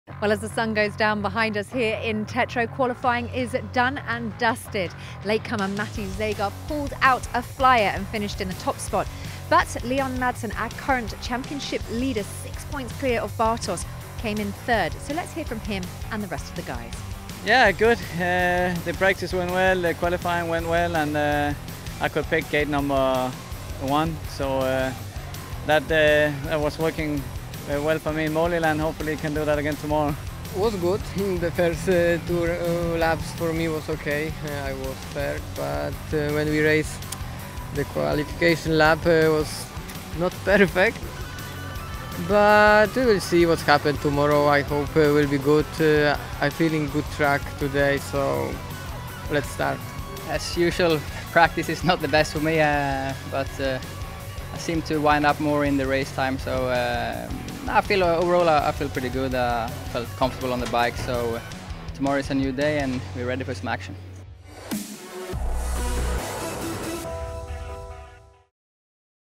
Qualifying Round-up | Aztorin German SGP
We caught up with the SpeedwayGP stars at Aztorin German SGP qualifying as Matej Zagar topped the charts in Teterow.